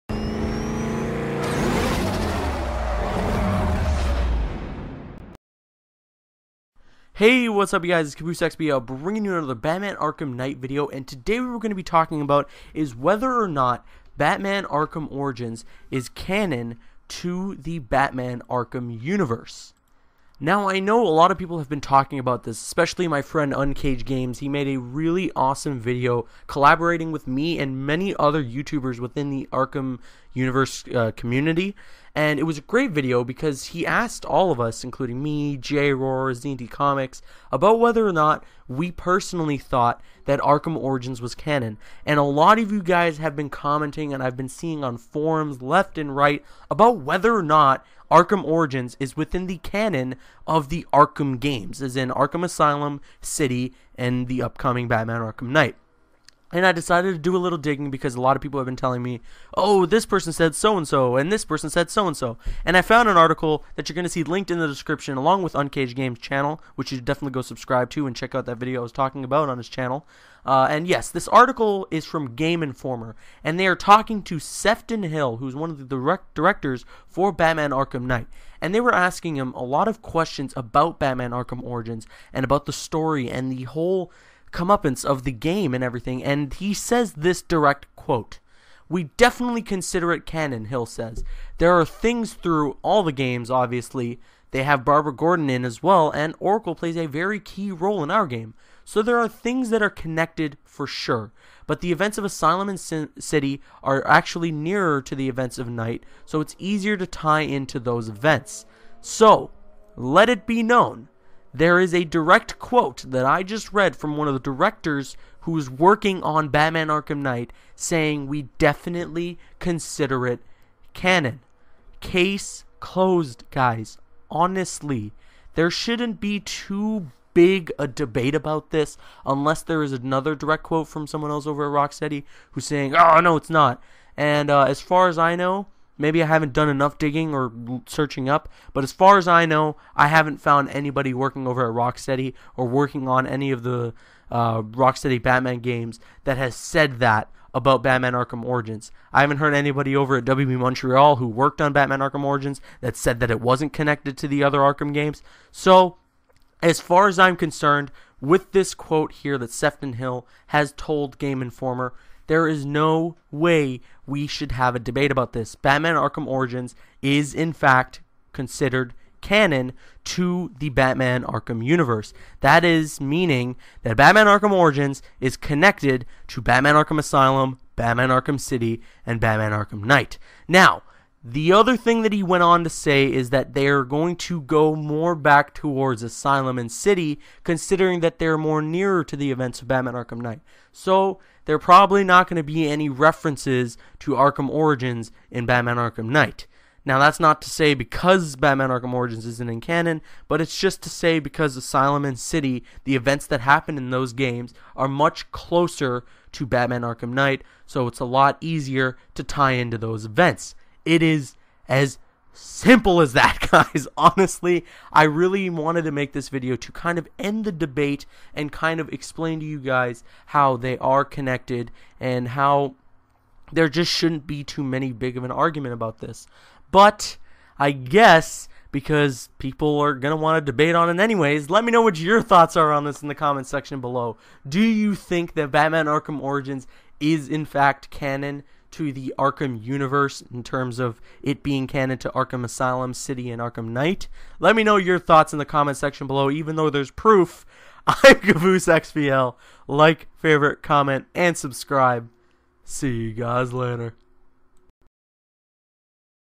Is Batman Arkham Origins Canon?
★:Be sure to follow Jessica who makes the EPIC thumbnails for my videos

Batman: Arkham Origins features an expanded Gotham City and introduces an original prequel storyline occurring several years before the events of Batman: Arkham Asylum and Batman: Arkham City. Taking place before the rise of Gotham City's most dangerous villains and assassins, the game showcases a young, raw, unrefined Batman as he faces a defining moment in his early career as a crime fighter that sets his path to becoming the Dark Knight. As the story unfolds, witness identities being formed and key relationships being forged.

Batman Arkham Knight features Scarecrow as he returns to Gotham City with a scheme to unite the Rogues Gallery and destroy the Batman once and for all. As the citizens of Gotham flee and criminal gangs take control of the city, Batman evens the odds taking to the streets in the legendary Batmobile to ignite the fight that will decide the future of the city that he is sworn to protect.